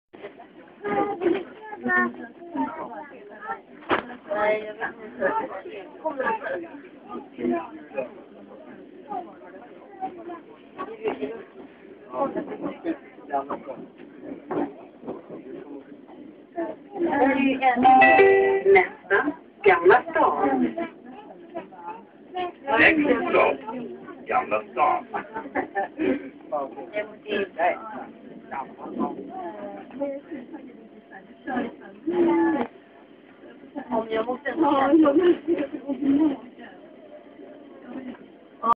Paseando en tren por Estocolmo
Llegando a la estacion Gamla Stan. La calidad no es muy buena debido a que fue filmado con el celular.